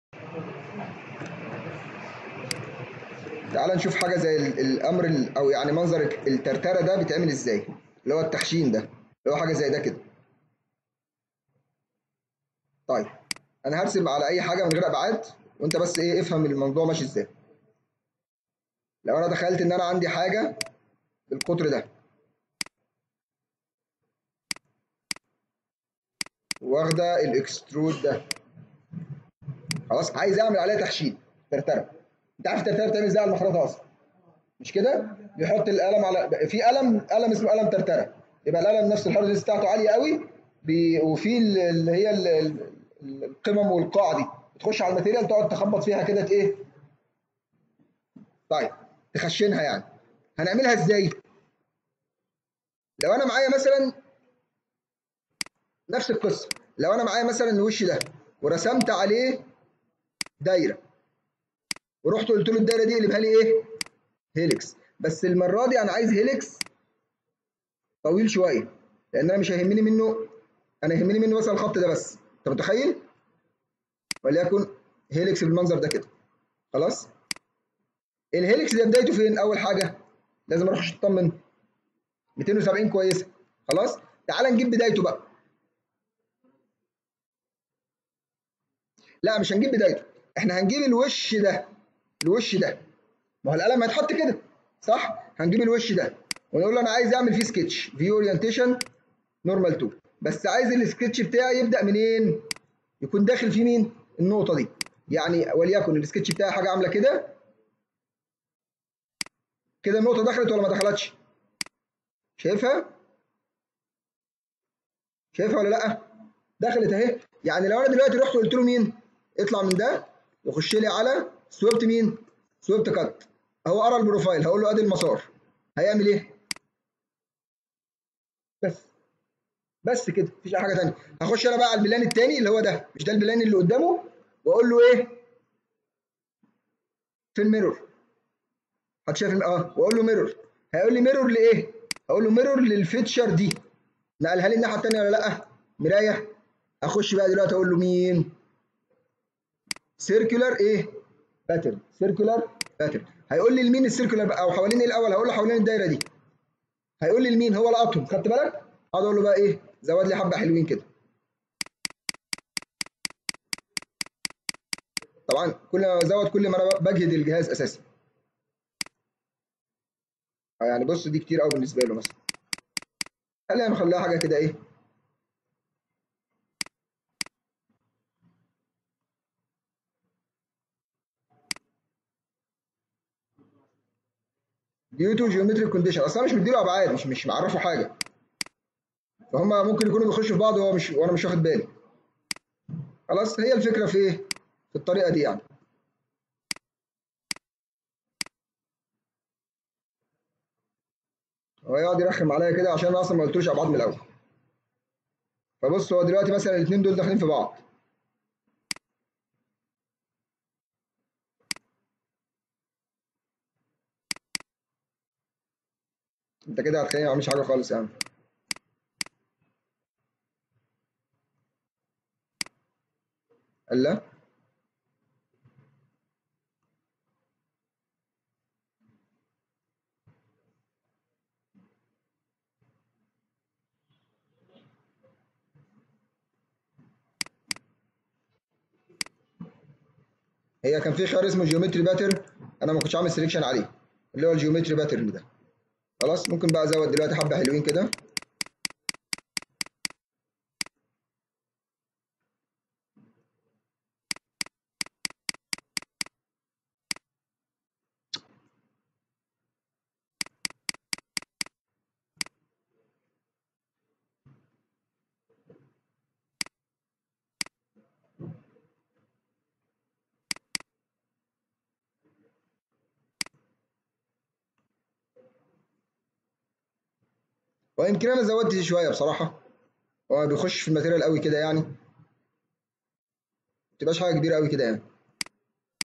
طريقة عمل الترتره على solidworks
الطريقه صح لكن انا اللى اخترت اتجاه ال mirror على plan غلط